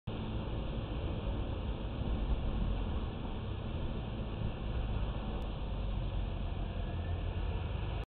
Finevu Cr500 HD Rear View Night Footage with 5% Rear window tint
Authorized Blackvue Distributor